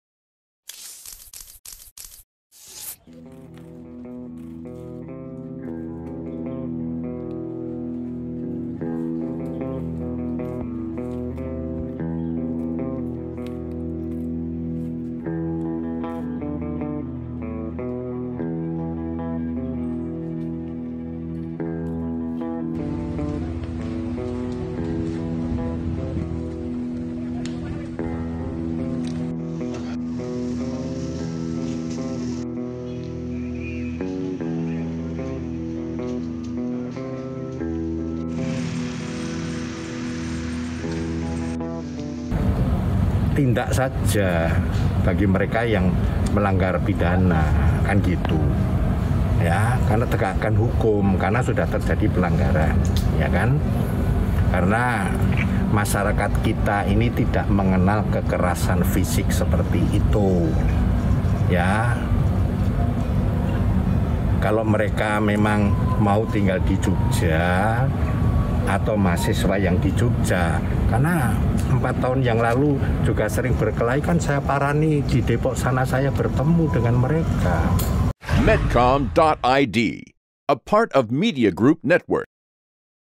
Kerusuhan di Sleman, Sultan HB X Minta Pelaku Ditindak Secara Hukum
Kerusuhan antarkelompok yang terjadi di Jalan Babarsari, Sleman, Yogyakata menuai kecaman keras dari Gubernur DIY Sri Sultan Hamengku Buwono X. Selain meminta aparat kepolisian menindak secara hukum, sultan juga mengingatkan pendatang jangan buat rusuh. Ia menyayangkan perselisihan antar kelompok kerap terjadi dan sultan harus turun tangan.

Sultan menegaskan kembali agar polisi menindak pelaku kerusuhan. Sultan berharap kelompok yang berseteru maupun siapa saja mahasiswa dari luar daerah yang tinggal di Yogyakarta bisa saling menghargai dan menghormati serta mengedapankan dialog jika terjadi kesalapaham. Hudha Sungsang/Metro TV.